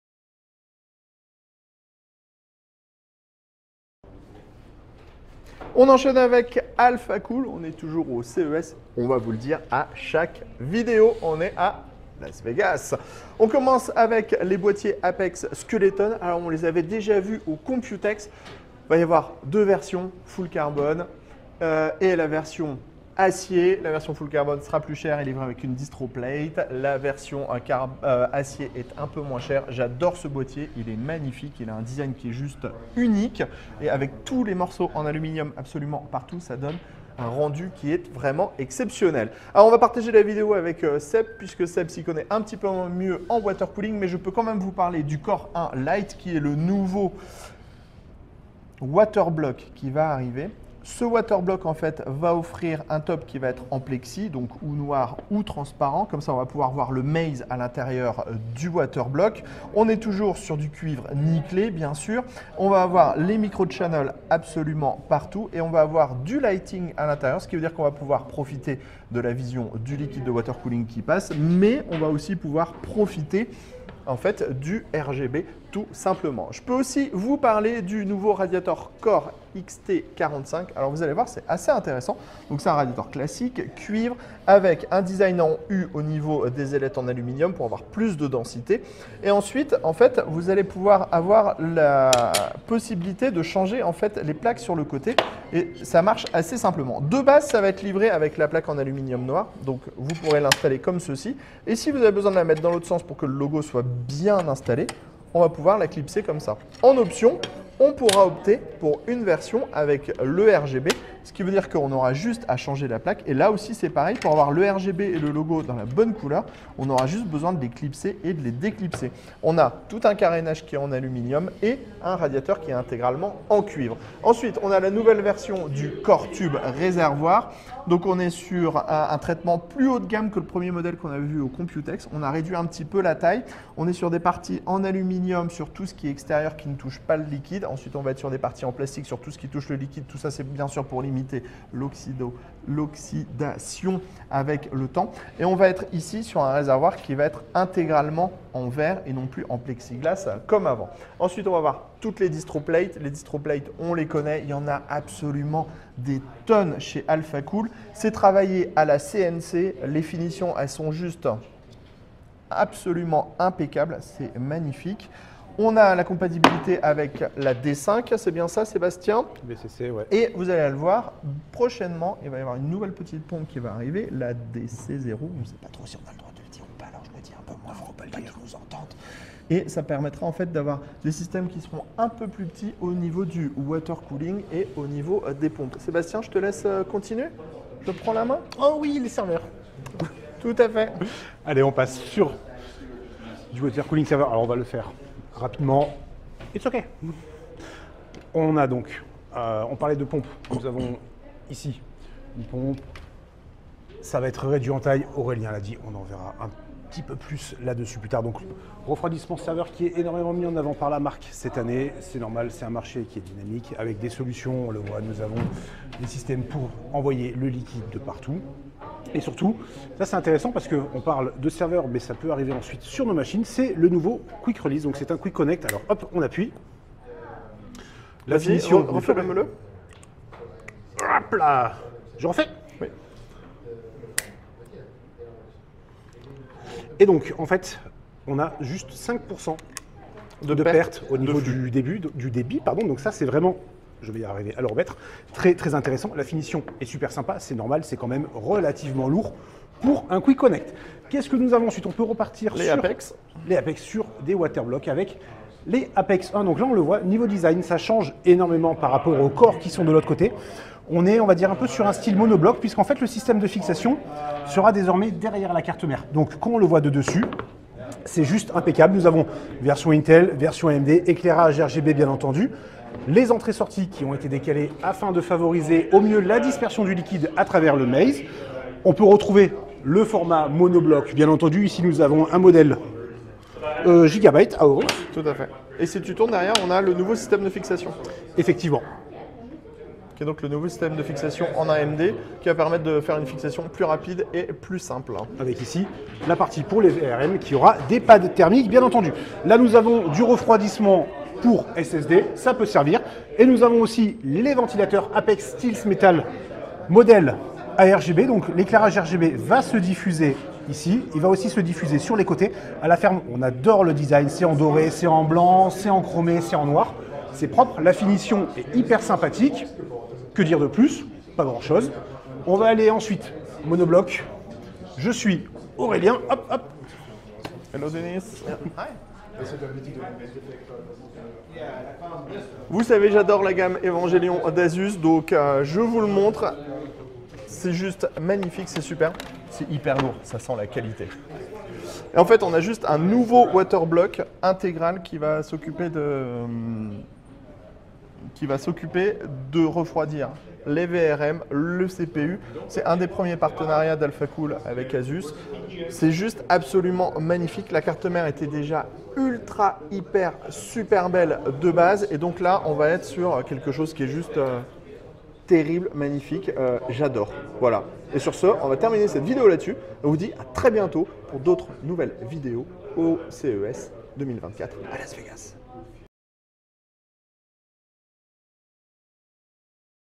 Alphacool fait le plein de watercooling au CES 2024
Chez Alphacool, on s'en doute, nous avons la dose de watercooling à découvrir avec les nouveaux waterblocks Core 1 LT et les APEX. Mais la marque ne s'arrête pas là avec des radaiteurs, des réservoirs, des boitiers et le tout est à découvrir dans cette vidéo.